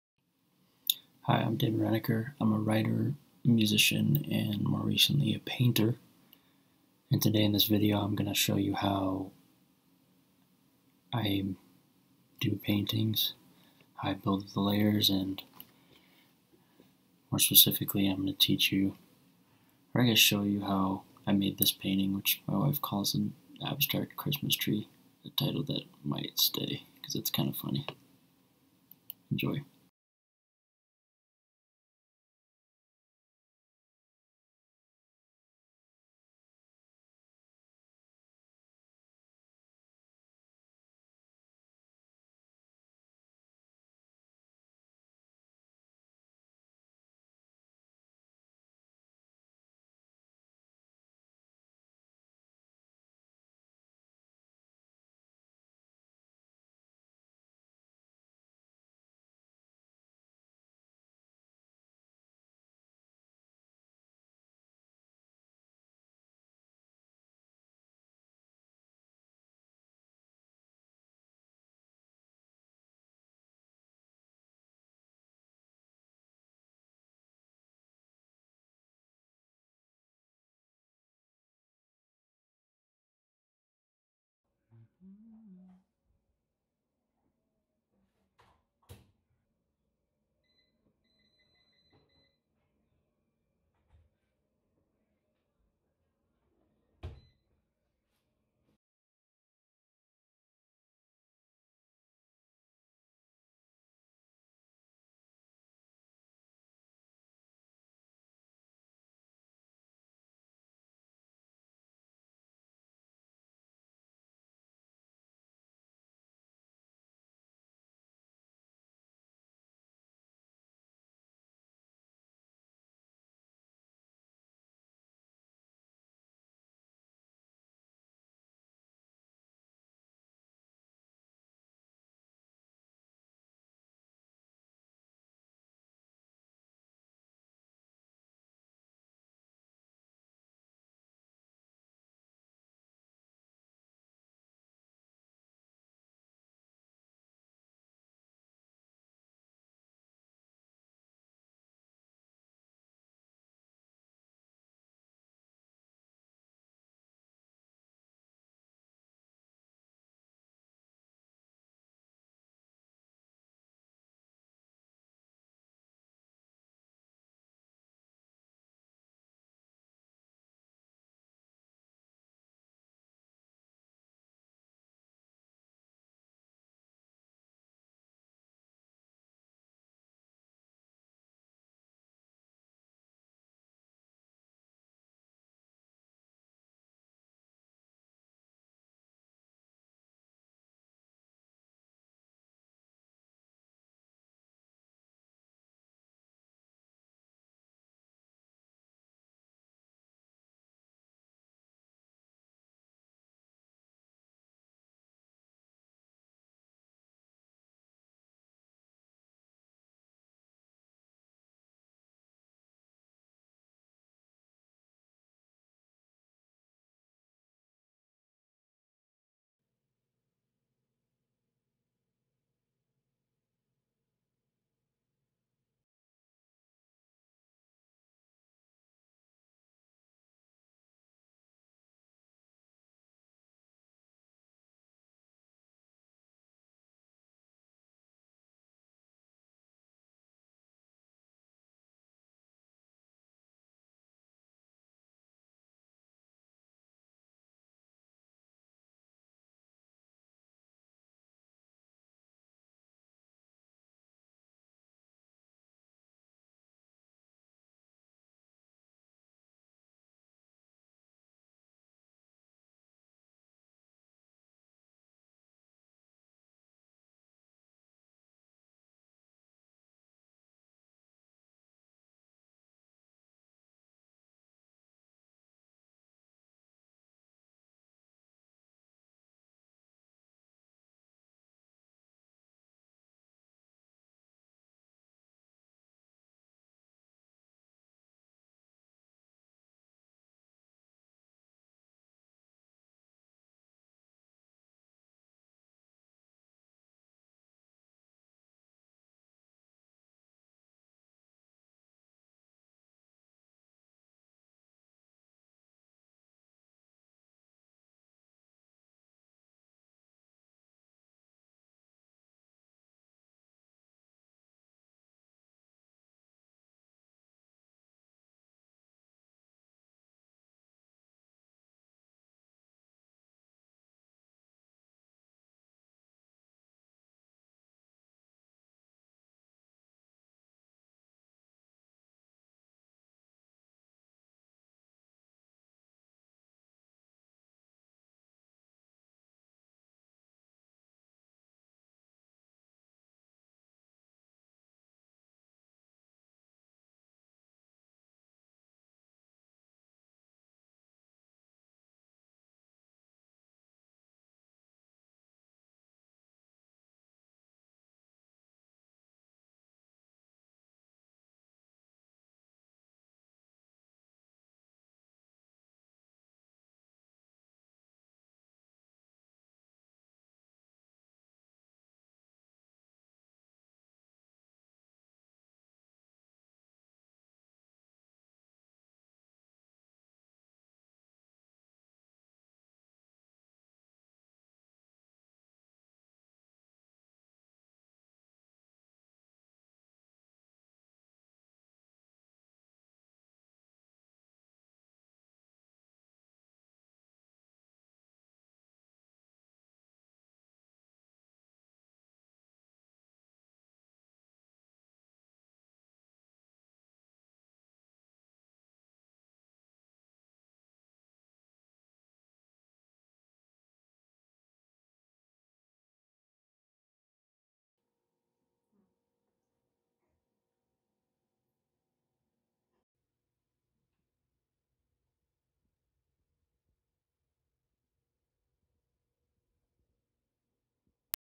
How I Paint | ABSTRACT ART in Acrylic | Step-by-Step The "Abstract Christmas Tree"
Here is a little get-to-know-you and tutorial packed into one. My wife has affectionately referred to this one as an "abstract Christmas tree" as a nod to her I'll keep it there.

Contents of the video:
0:00 Intro
0:01 I'm Damon
0:30 base layer
0:41 2nd layer (gesso)
1:24 maté break
2:43 texture
3:46 signature
4:37 green - the fun part
5:25 red - accent color
6:58 showing it off